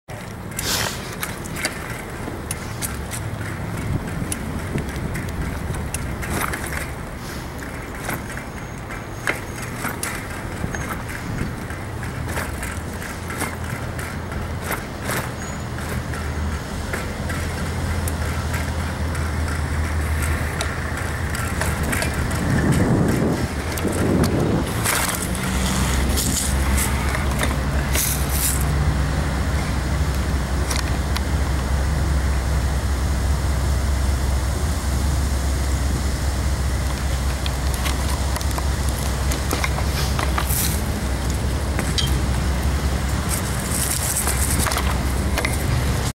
2013 10 24 Robin Hooding Keene x010
Linda and Jane try to find expired meters as Garret fills them. Bradford Hutchingson acts crazy and juvenile and usual, following Garret at uncomfortable distances despite repeated requests for space. AKPF supporters and fans much official DPRK Aqua Keene Parking Force #1